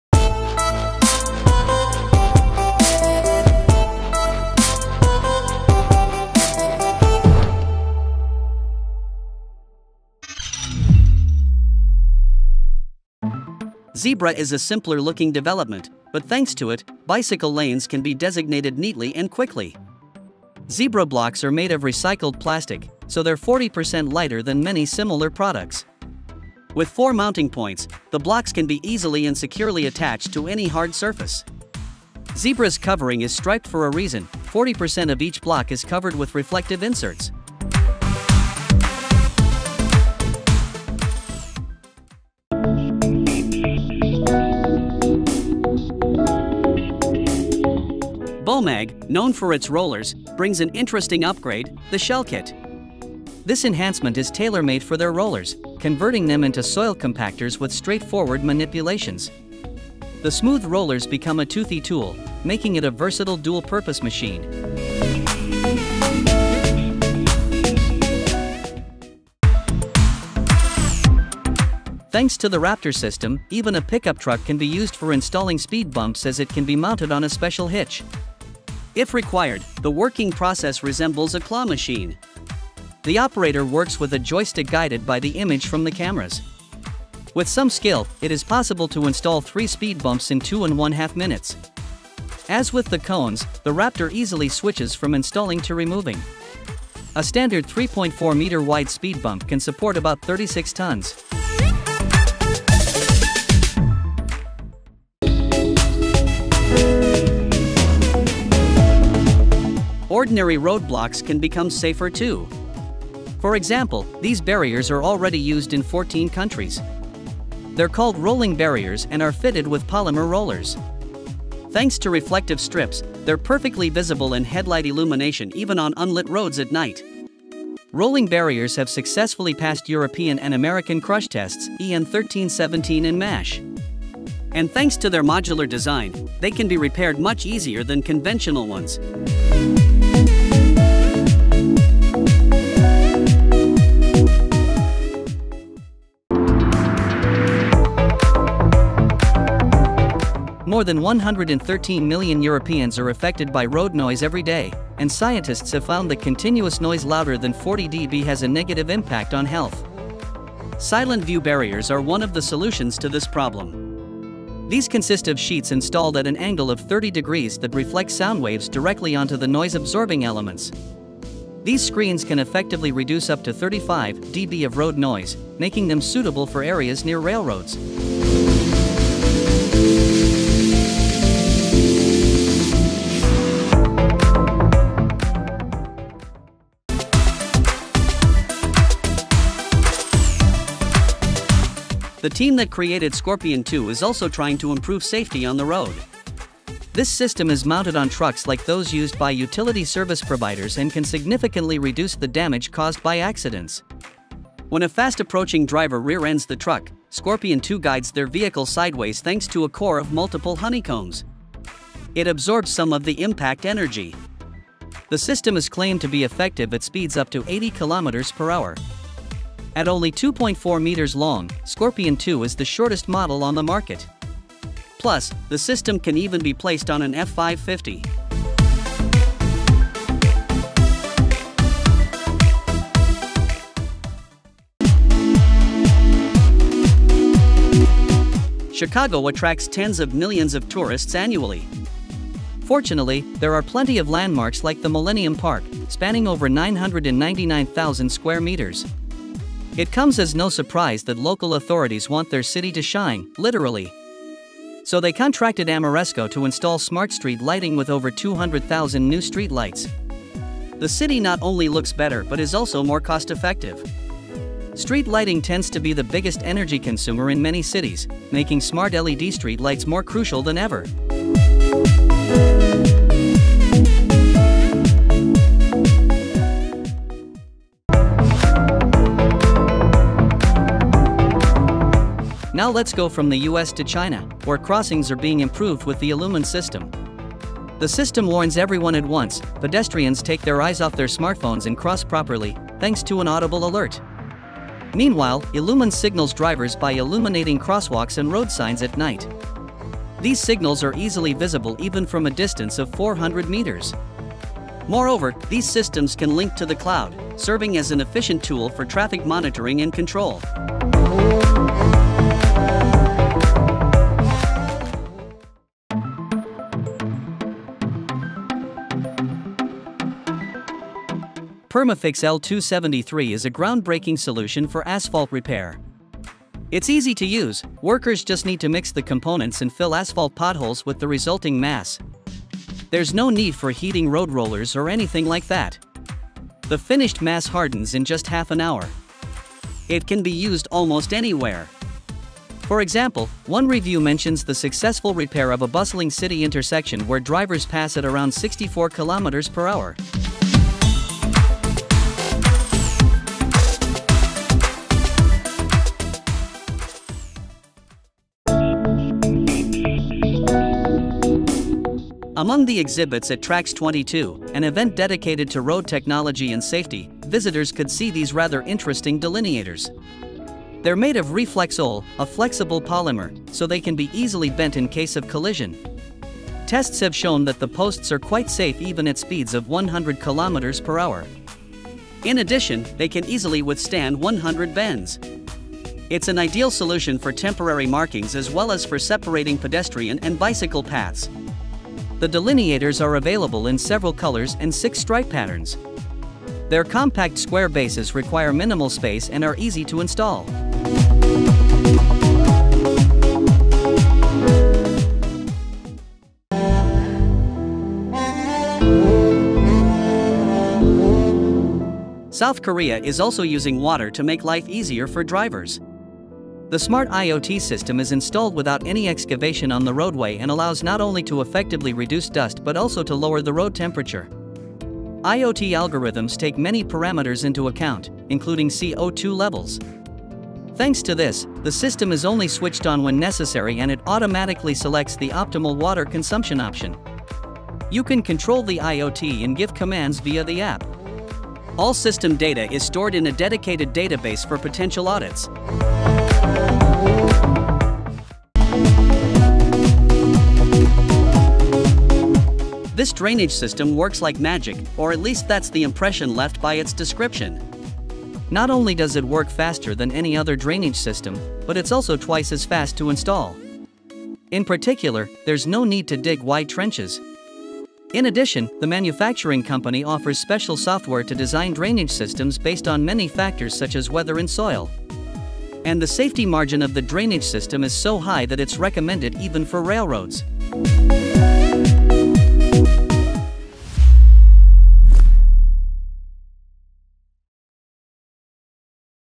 ROAD INNOVATION THAT WILL BLOW YOUR MIND
00:13 - Zebra® ZERO
00:42 - BOMAG Shell Kit
01:09 - RAPTOR Rover
01:45 - ETI Rolling Guardrail Barrier
02:26 - Soundstop SilentView
03:06 - Scorpion II® METRO
03:54 - Ameresco Smart LED
04:37 - ELLUMIN Intelligent Pedestrian System
05:15 - Meon PermaFyx L273
05:58 - Neopolitan™ Delineator Post
06:46 - MarkWay
07:28 - Geofabrics Megaflo® Green